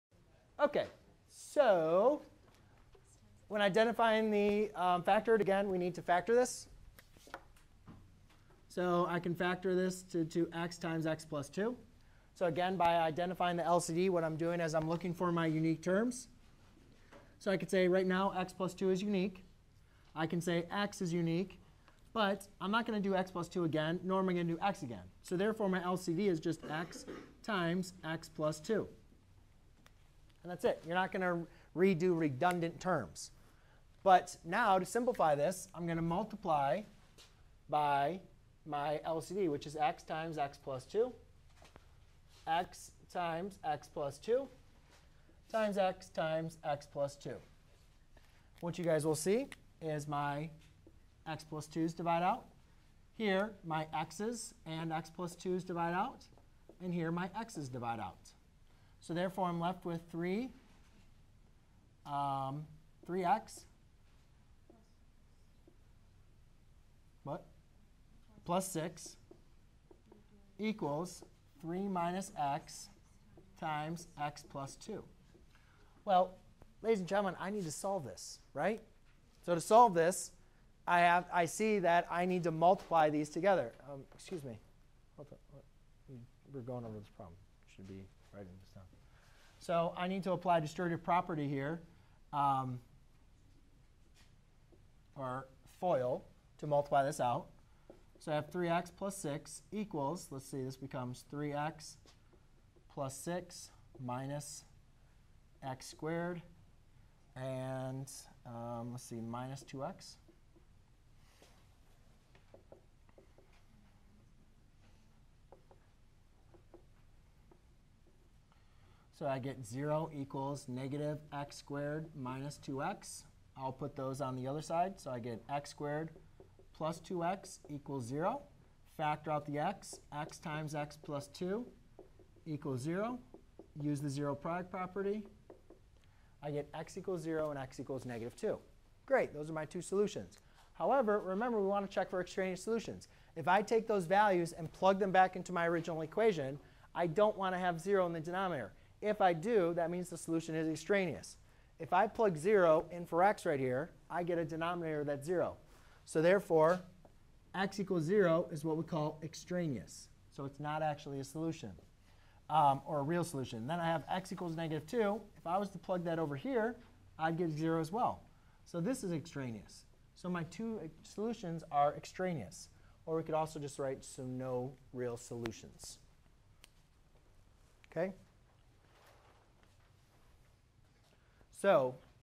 Solve a rational equation with extraneous solutions
👉 Learn how to solve rational equations. A rational expression is an expression in the form of a fraction where the numerator and/or the denominator are/is an algebraic expression. There are many ways to solve rational expressions, one of the ways is by multiplying all the individual rational expressions by the lowest common multiply (LCM) of the denominators. This eliminates the denominators and then we can evaluate the resulting equation accordingly.

It is very helpful to check your results by substituting them into the original equation to see if they are true. This also helps to identify the extraneous solutions.  An extraneous solution is a solution that is obtained during the course of solving an equation but which does not hold true for the original equation.